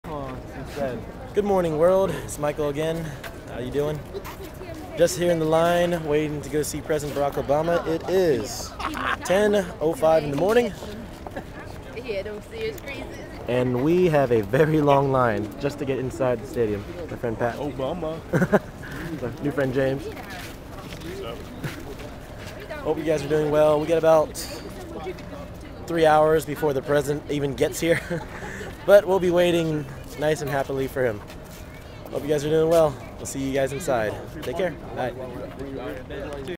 Waiting in line to get in Reynolds to see President Barack Obama
Outside with thousands of other people waiting to get inside to see the President later that day.